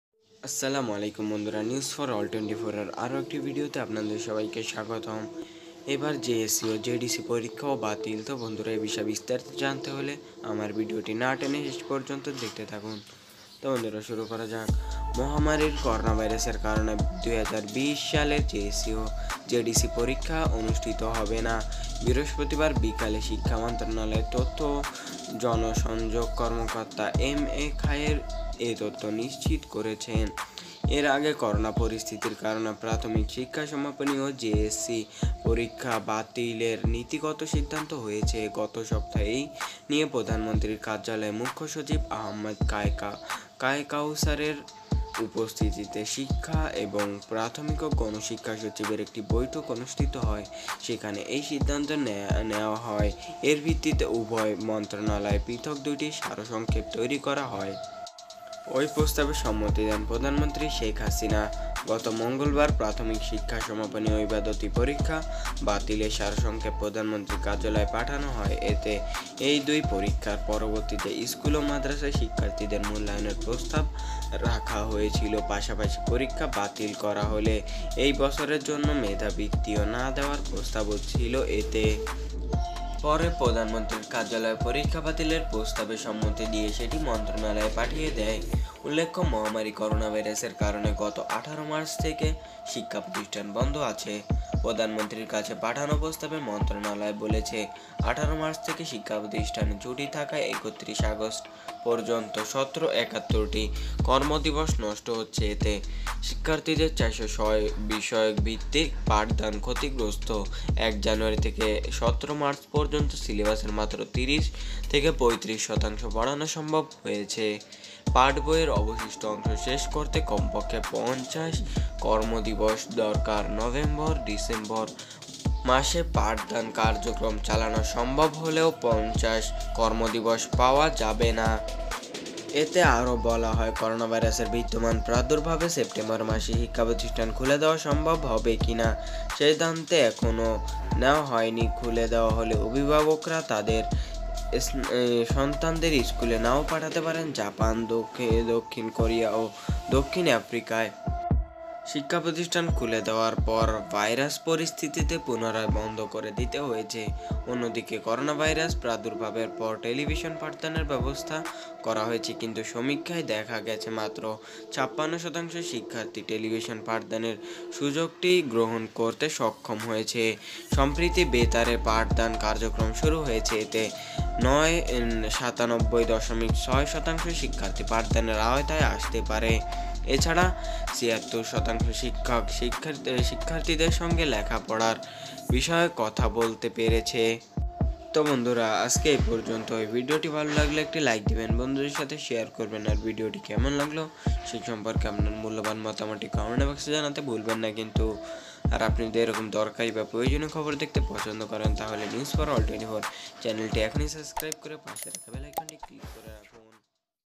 এবার জেএসসি-জেডিসি পরীক্ষাও বাতিল ||This time JSC-JDC examination was also canceled||News for all 24
এবার জেএসসি-জেডিসি পরীক্ষাও বাতিল

মহামারীর করোনাভাইরাসের কারণে ২০২০ সালের জেএসসি ও জেডিসি পরীক্ষা অনুষ্ঠিত হবে না।

বৃহস্পতিবার বিকালে শিক্ষা মন্ত্রণালয়ের তথ্য ও জনসংযোগ কর্মকর্তা এম এ খায়ের যুগান্তরকে এ তথ্য নিশ্চিত করেছেন।

এর আগে করোনা পরিস্থিতির কারণে প্রাথমিক শিক্ষা সমাপনী ও জেএসসি পরীক্ষা বাতিলের নীতিগত সিদ্ধান্ত হয়েছে। গত সপ্তাহে এ নিয়ে প্রধানমন্ত্রীর কার্যালয়ে মুখ্য সচিব আহমদ কায়কাউসের উপস্থিতিতে শিক্ষা এবং প্রাথমিক ও গণশিক্ষা সচিবের একটি বৈঠক অনুষ্ঠিত হয়। সেখানে এই সিদ্ধান্ত নেয়া হয়। এর ভিত্তিতে উভয় মন্ত্রণালয়ে পৃথক দুটি সারসংক্ষেপ তৈরি হয়।

ওই প্রস্তাবে সম্মতি দেন প্রধানমন্ত্রী শেখ হাসিনা।

গত মঙ্গলবার প্রাথমিক শিক্ষা সমাপনী ও ইবতেদায়ি পরীক্ষা বাতিলে সারসংক্ষেপ প্রধানমন্ত্রী কার্যালয়ে পাঠানো হয়। এতে এ দুই পরীক্ষার পরিবর্তে স্কুল ও মাদ্রাসায় শিক্ষার্থীদের মূল্যায়নের প্রস্তাব রাখা হয়েছিল। পাশাপাশি পরীক্ষা বাতিল করা হলে এ বছরের জন্য মেধাবৃত্তিও না দেয়ার প্রস্তাবও ছিল এতে।

পরে প্রধানমন্ত্রীর কার্যালয় পরীক্ষা বাতিলের প্রস্তাবে সম্মতি দিয়ে সেটি মন্ত্রণালয়ে পাঠিয়ে দেয়।

উল্লেখ্য, মহামারী করোনাভাইরাসের কারণে গত ১৮ মার্চ থেকে শিক্ষাপ্রতিষ্ঠান বন্ধ আছে। প্রধানমন্ত্রীর কাছে পাঠানো প্রস্তাবে মন্ত্রণালয় বলেছে– ১৮ মার্চ থেকে শিক্ষাপ্রতিষ্ঠানে ছুটি থাকায় ৩১ আগস্ট পর্যন্ত ৭১টি কর্মদিবস নষ্ট হচ্ছে। এতে শিক্ষার্থীদের ৪০৬ বিষয়ভিত্তিক পাঠদান ক্ষতিগ্রস্ত। ১ জানুয়ারি থেকে ১৭ মার্চ পর্যন্ত সিলেবাসের মাত্র ৩০ থেকে ৩৫ শতাংশ পড়ানো সম্ভব হয়েছে। পাঠ্যবইয়ের অবশিষ্ট অংশ শেষ করতে কমপক্ষে ৫০ কর্মদিবস দরকার। নভেম্বর-ডিসেম্বর মাসে পাঠদান কার্যক্রম চালানো সম্ভব হলেও ৫০ কর্মদিবস পাওয়া যাবে না।

এতে আরও বলা হয়, করোনাভাইরাসের বিদ্যমান প্রাদুর্ভাবে সেপ্টেম্বর মাসে শিক্ষাপ্রতিষ্ঠান খুলে দেয়া সম্ভব হবে কিনা সে সিদ্ধান্ত এখনও নেয়া হয়নি। খুলে দেয়া হলে অভিভাবকরা তাদের সন্তানদের স্কুলে নাও পাঠাতে পারেন। জাপান, দক্ষিণ কোরিয়া ও দক্ষিণ আফ্রিকায় শিক্ষাপ্রতিষ্ঠান খুলে দেয়ার পর ভাইরাস পরিস্থিতিতে পুনরায় বন্ধ করে দিতে হয়েছে। অন্যদিকে করোনাভাইরাস প্রাদুর্ভাবের পর টেলিভিশনে পাঠদানের ব্যবস্থা করা হয়েছে। কিন্তু সমীক্ষায় দেখা গেছে, মাত্র ৫৬ শতাংশ শিক্ষার্থী টেলিভিশন পাঠদানের সুযোগটি গ্রহণ করতে সক্ষম হয়েছে।

সম্প্রতি বেতারে পাঠদান কার্যক্রম শুরু হয়েছে। এতে ৯৭ দশমিক ৬ শতাংশ শিক্ষার্থী পাঠদানের আওতায় আসতে পারে। এ ছাড়া ৭৬ শতাংশ শিক্ষক-শিক্ষার্থীদের সঙ্গে লেখাপড়ার বিষয়ে কথা বলতে পেরেছে।